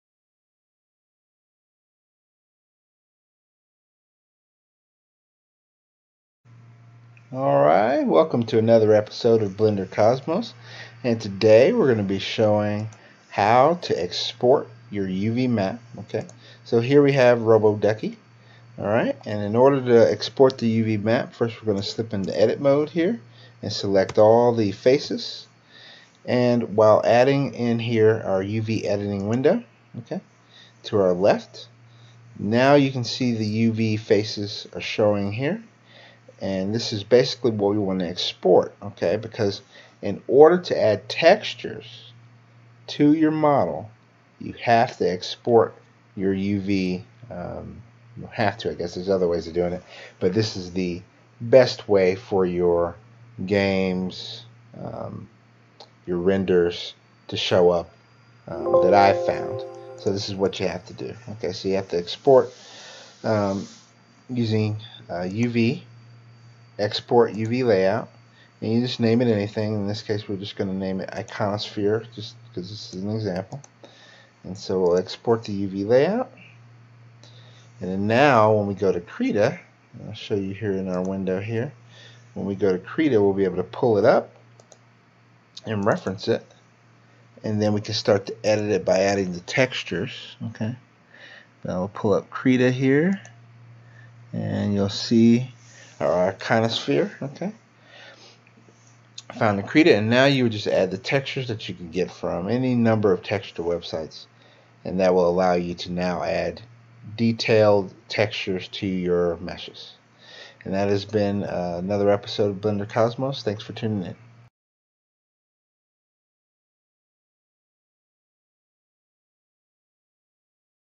EXPORTING UV MAPS FROM BLENDER TO KRITA
Hey guys, here's another video showing how to export your UV maps from the meshes you've created in blender to another program - in this example we use Krita. With this we can now add detailed textures to our 3D model and get it ready for its debut in our games and cinematography.